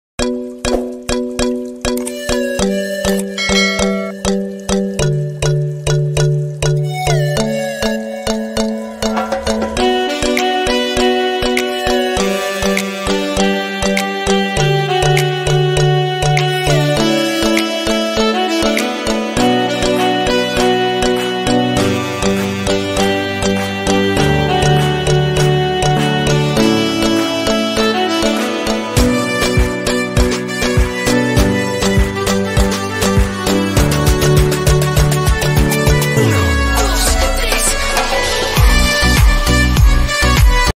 OMG 😱 Bigg Boss Priyanka Reacts to my leaf art made for her | Bigg Boss Tamil season 5
OMG 😱 Bigg Boss Priyanka Reacts to my leaf art made for her | Bigg Boss Tamil season 5 | The Arts I Love |

@Priyanka Deshpande